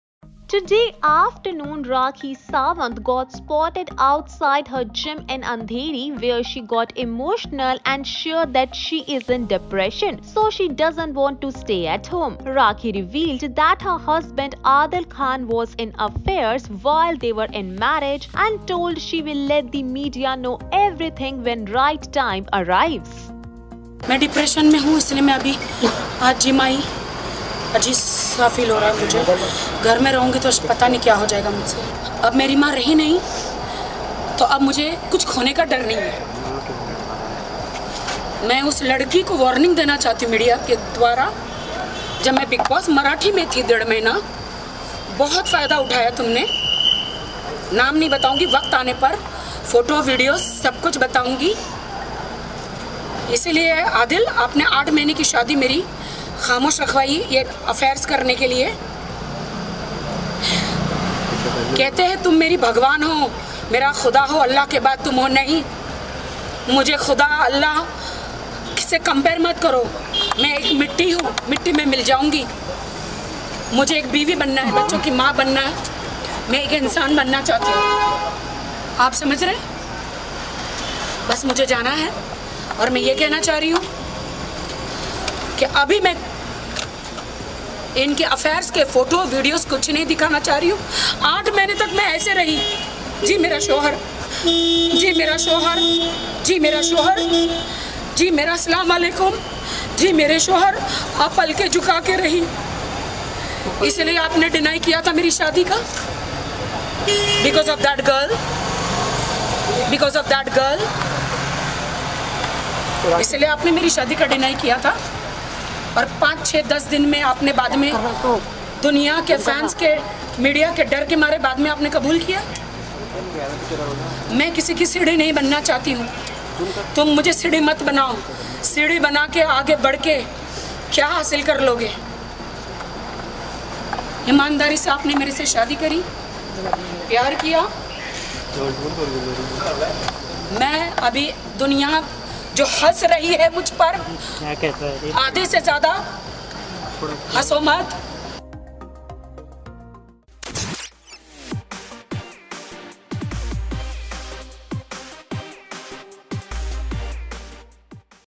Rakhi Sawant Spotted At Outside Gym In Andheri I Boogle Bollywood
Copyrights@BoogleBollywood